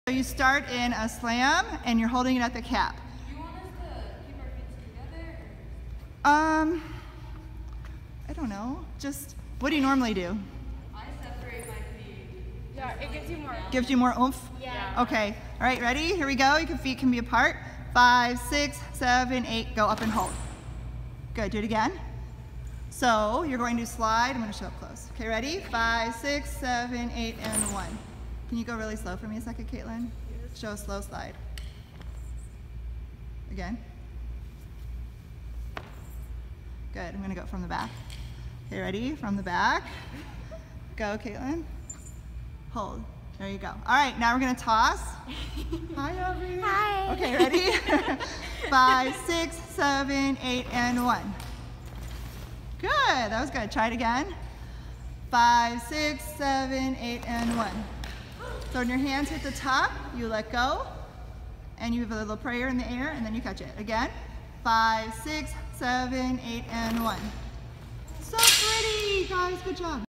Prayer Toss
Prayer toss taught at mini camp 2021